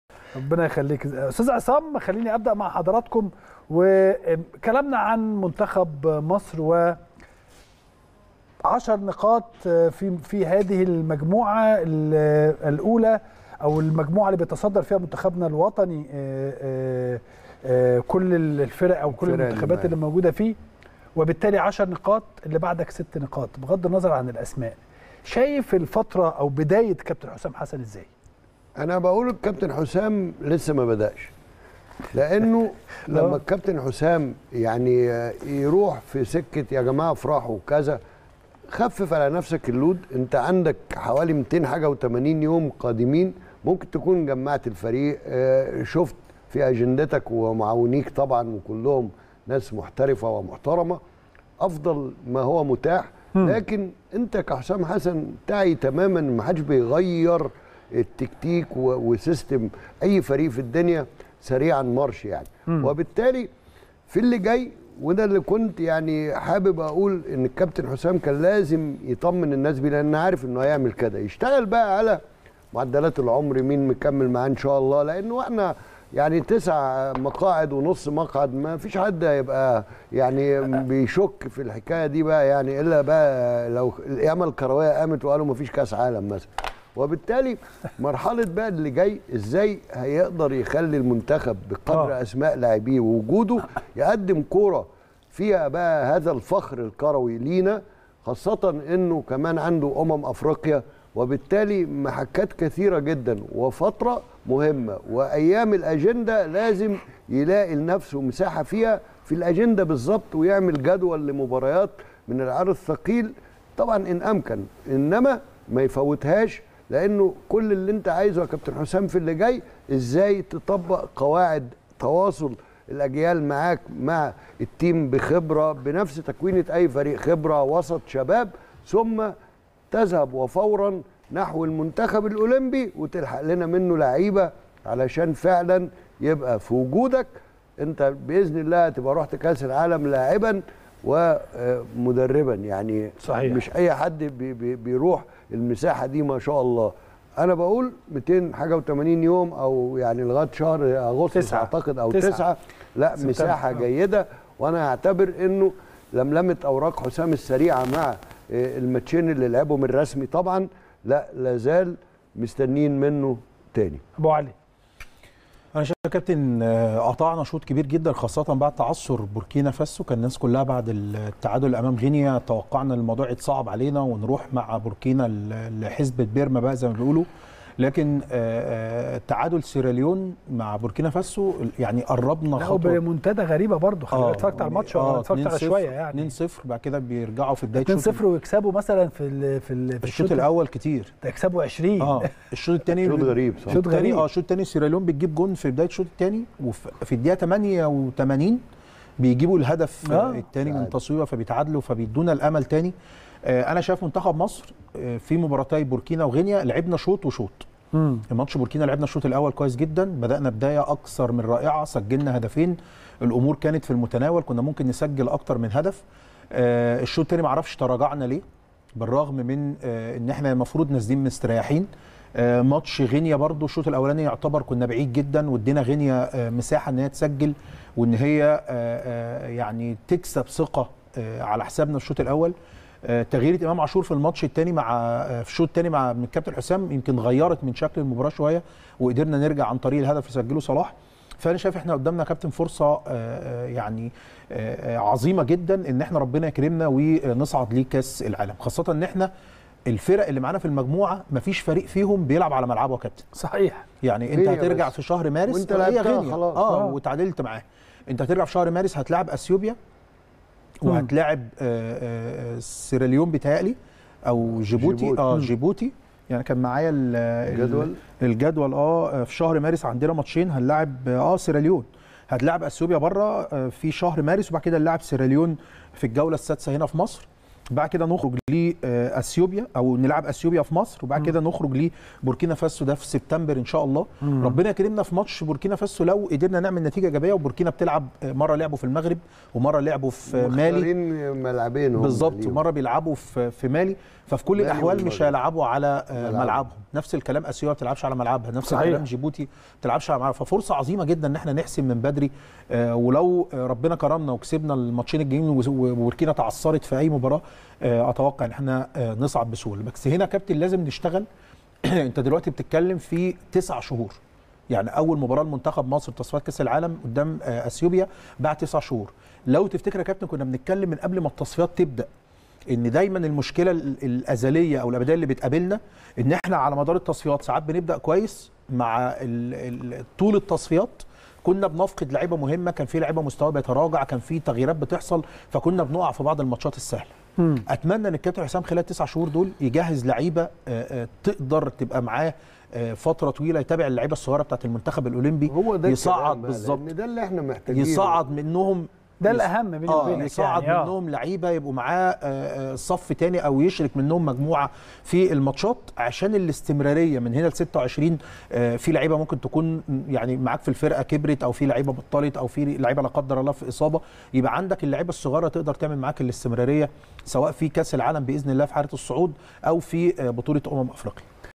"عصام شلتوت ومحمد أبو علي" ورؤيتهم الفنية لبداية حسام_حسن مع المنتخب في تصفيات
النقاد الرياضيين "عصام شلتوت ومحمد أبو علي" ورؤيتهم الفنية لبداية حسام_حسن مع المنتخب في تصفيات